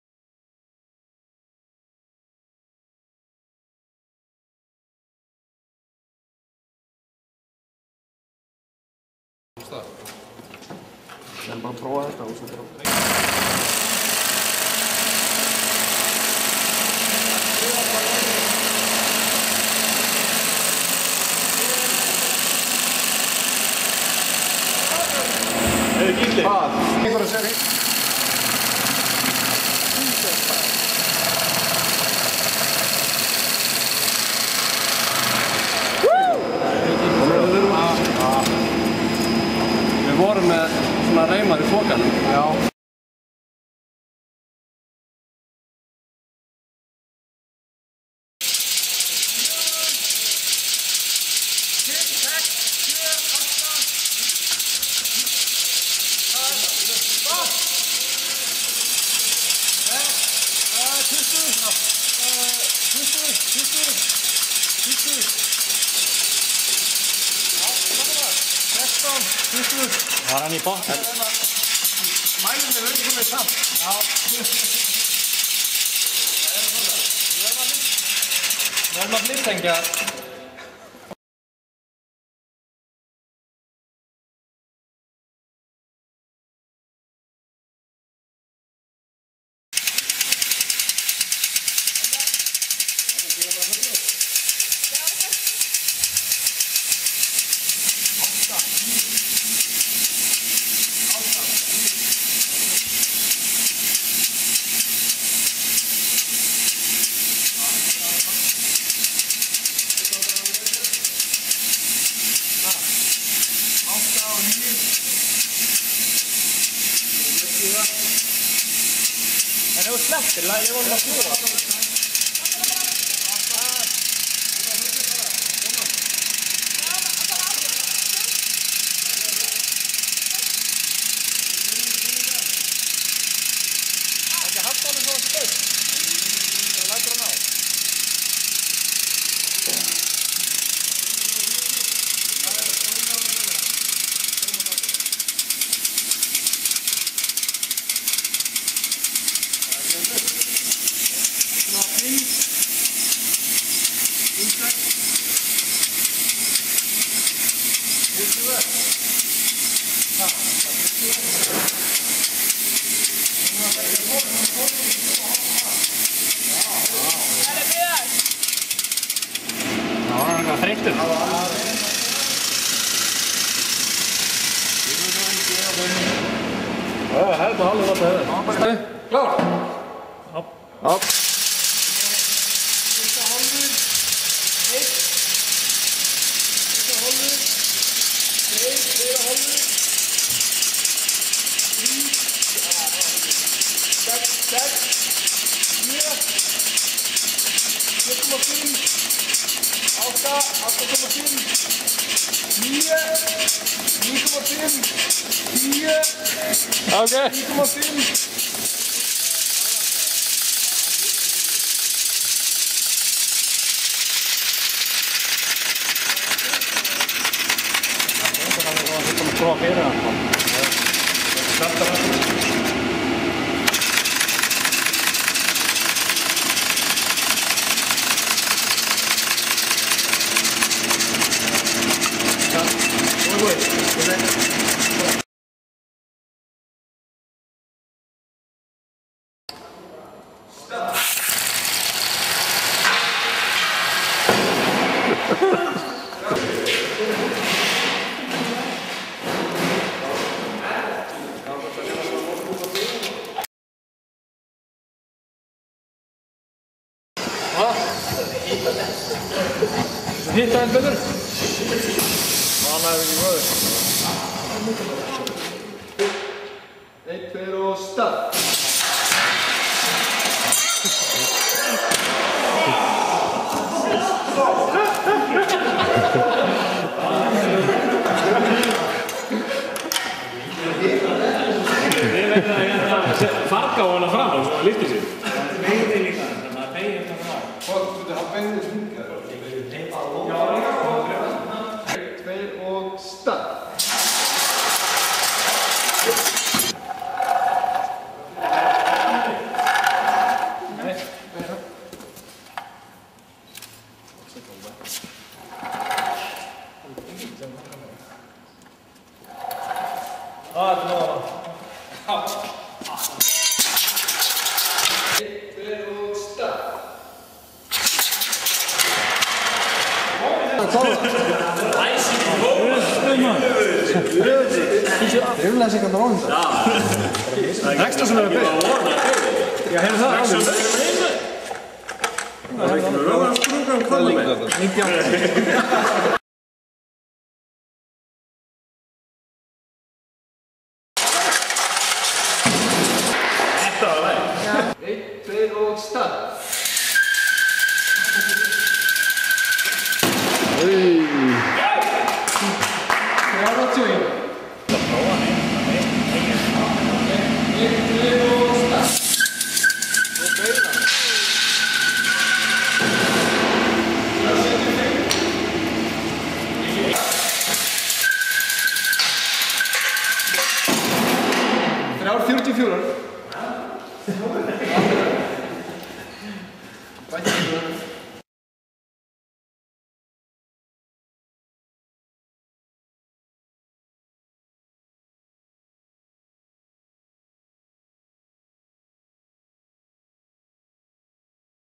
Mótorsmíði 2008 Háskólinn í Reykjavík
mótorsmíði HR-inga 2008, smíða mótor aflprófa hann, smíða bíl og tíma taka.

Students inventing and designing a compressed air motor, then the torque was measured. Afterwards cars were made and raced.